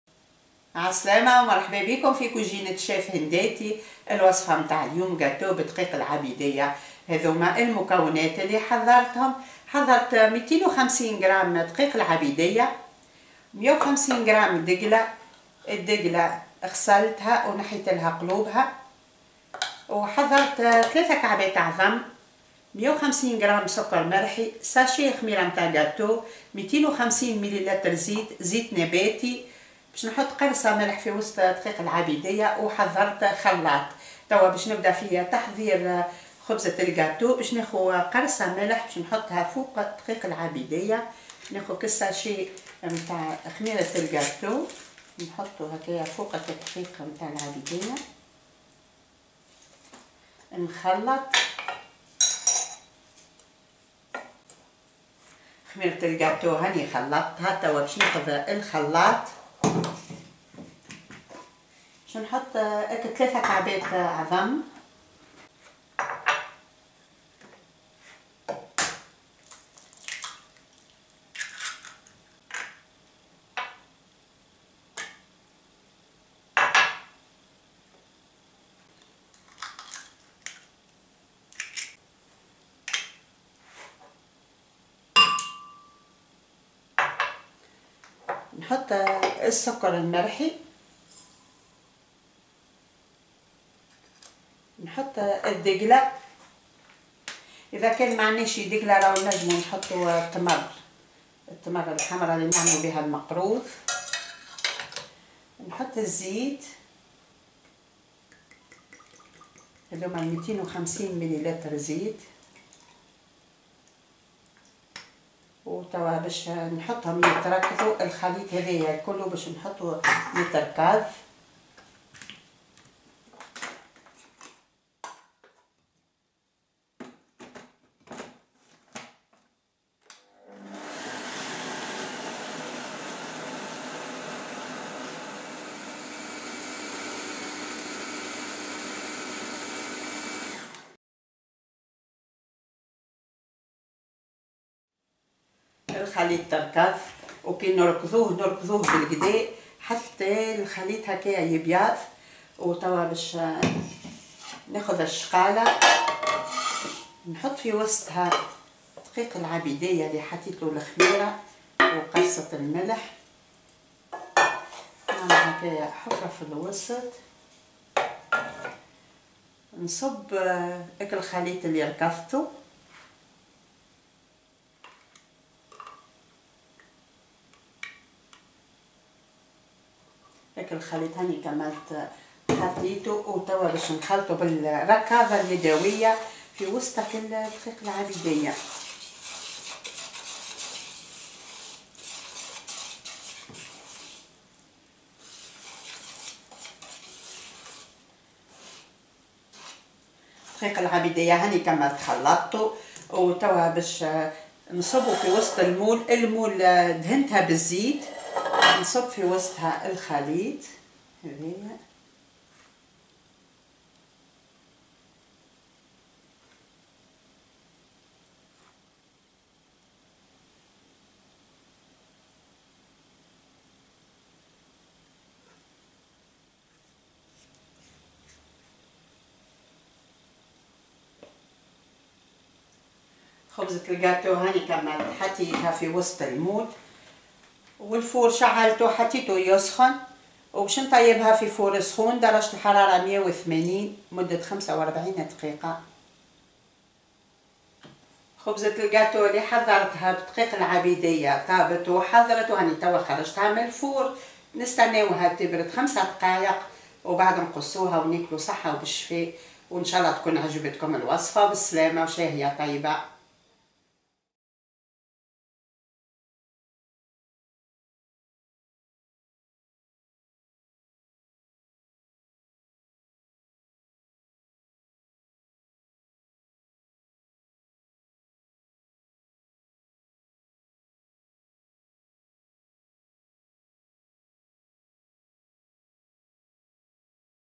Gâteau au mais recette sans gluten savoureuse, délicieuse et facile قاتو بدقيق العابدية بدون غلوتان
Recette savoureuse,délicieuse sans gluten séduit les amoureux de la cuisine
Ingrédients:
250g de farine de mais
250ml huile végétale
3 oeufs
150 g de sucre
150 g de dattes
1 s de levure chimique
1 pincée de sel
Bon appétit